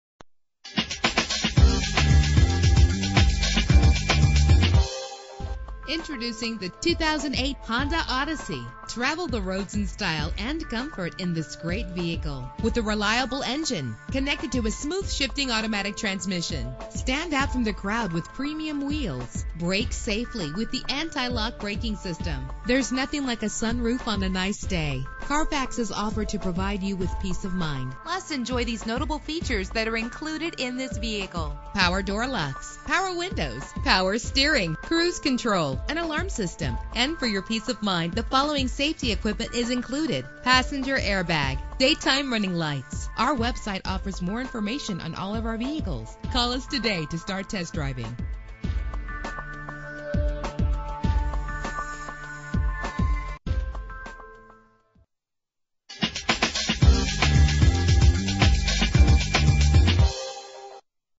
2008 Honda Odyssey - Manheim PA
Year: 2008
Make: Honda
Model: Odyssey
Trim: EX-L Minivan 4D
Engine: 6 cylinder VTEC 3.5 liter
Transmission:
Color: Burgundy
Mileage: 39806
Address:
14 Anthony Drive